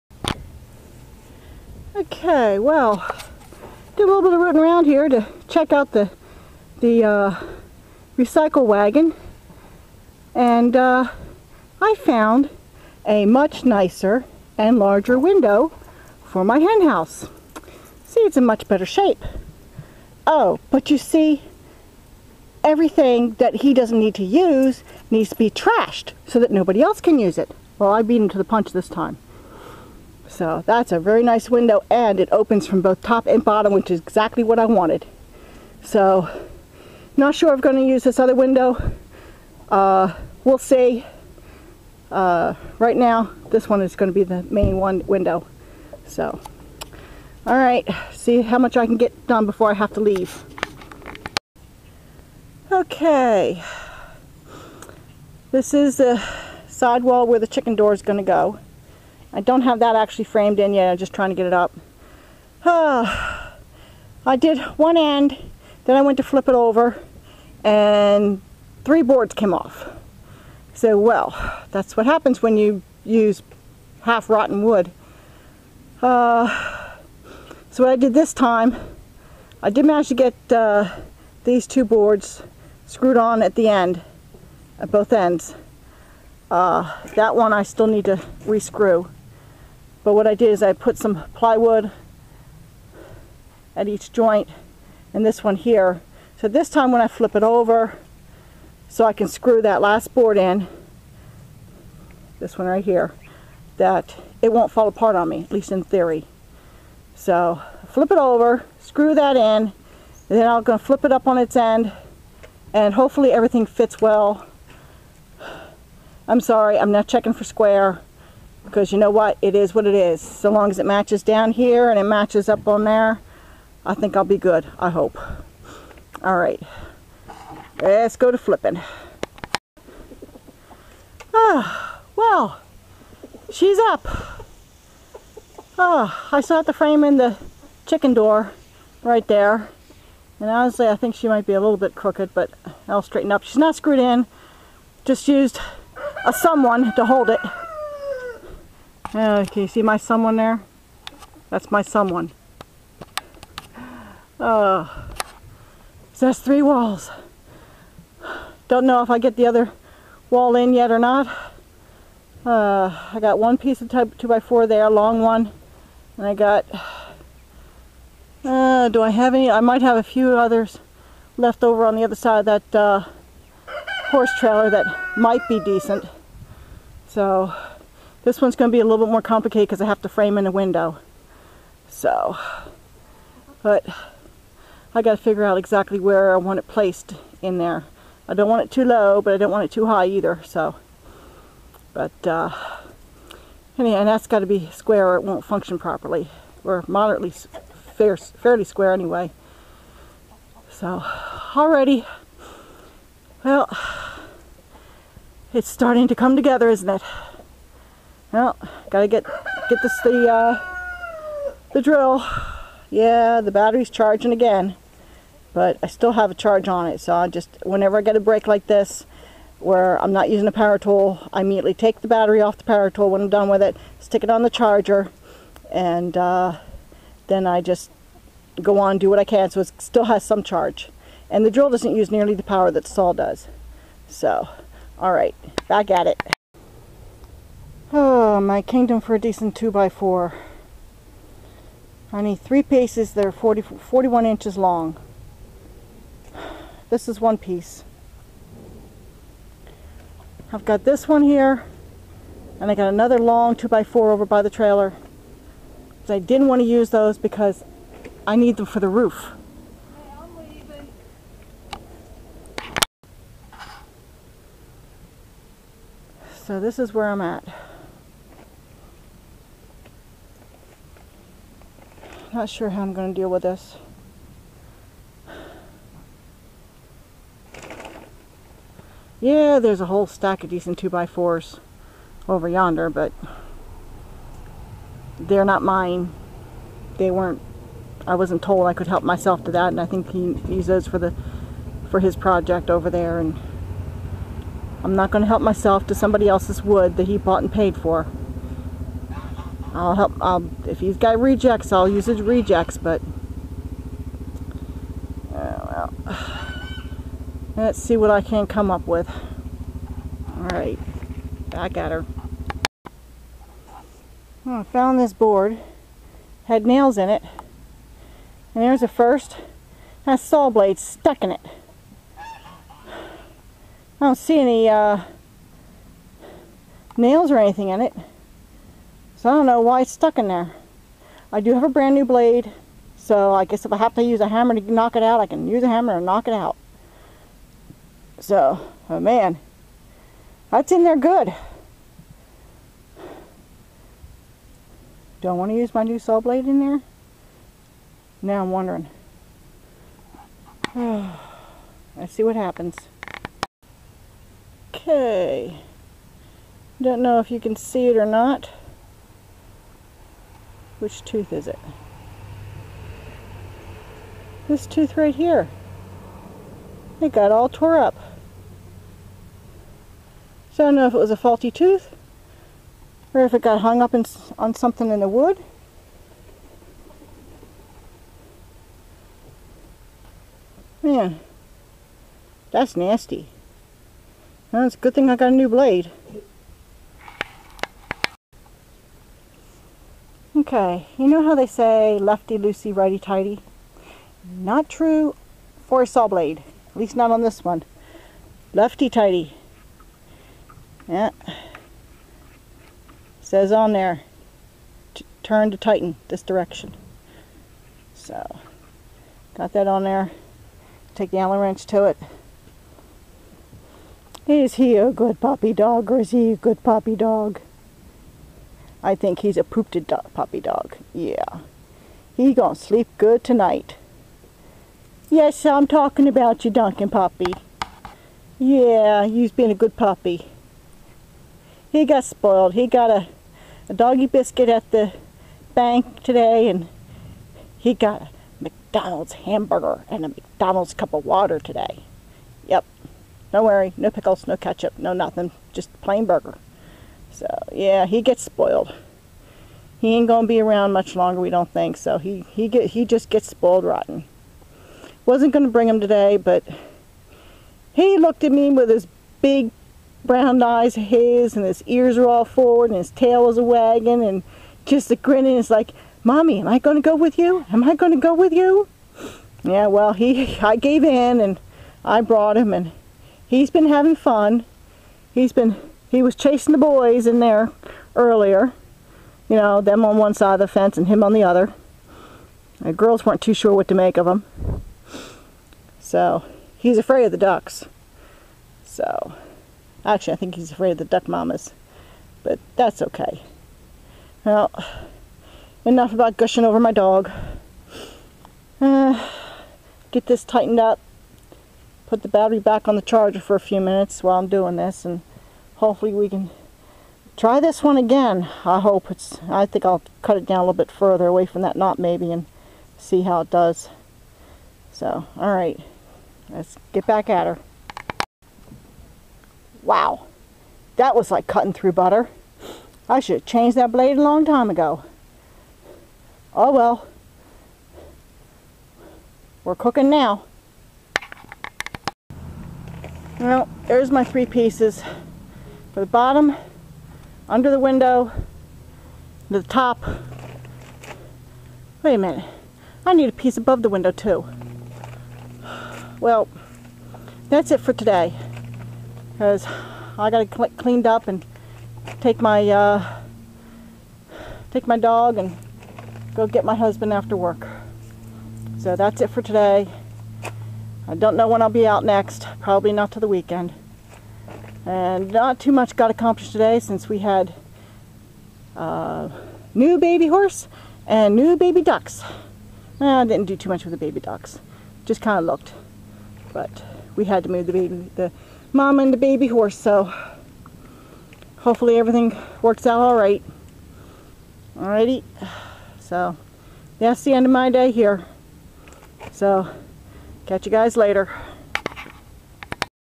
Another Wall and More
Didn't get as much accomplished as I would have liked. Had a bit of excitement as you saw on the previous video.  However, because of it I got a nice score on a better window!
Trying to not have to buy any wood, but I am quickly running out of 2x4's that are decent enough to use.